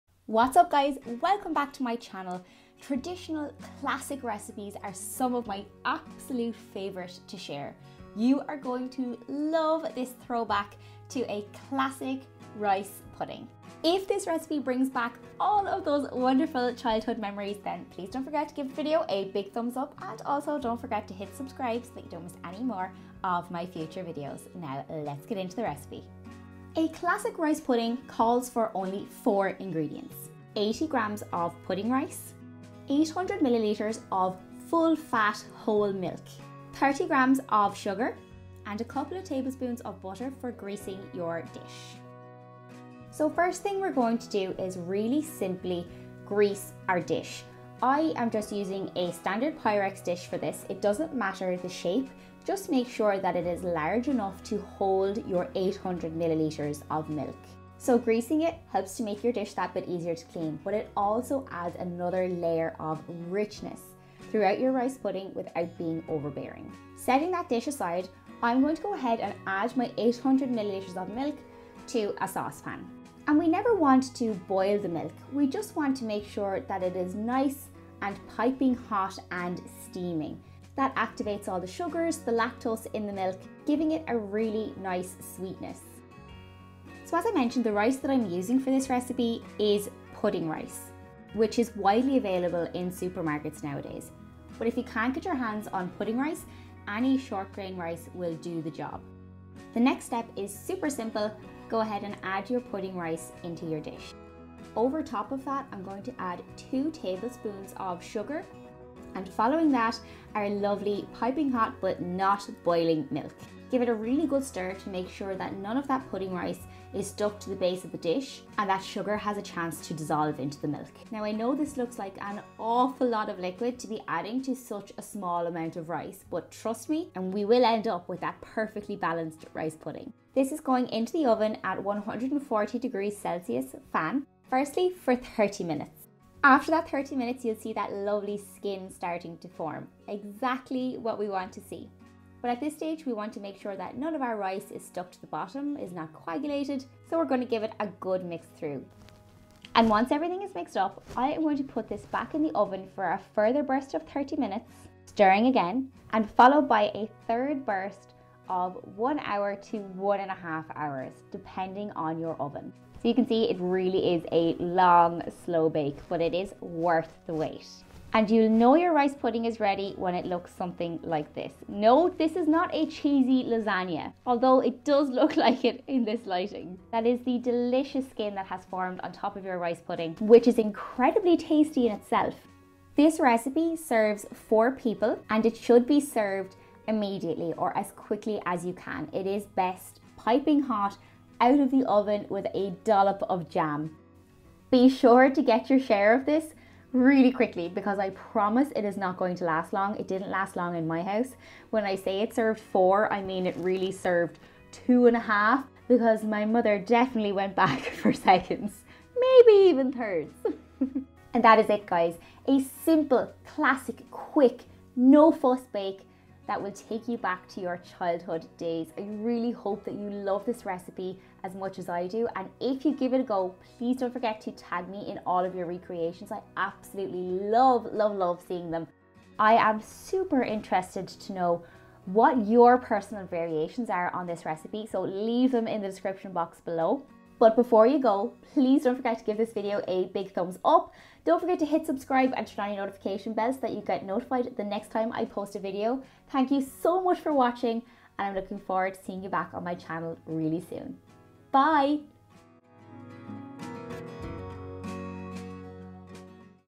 HOW TO MAKE CLASSIC RICE PUDDING| 4 Ingredients
Classic ricepudding is a childhood favourite that brings back so many memories! With just 4 simple ingredients you can recreate all the good memories and the amazing flavour!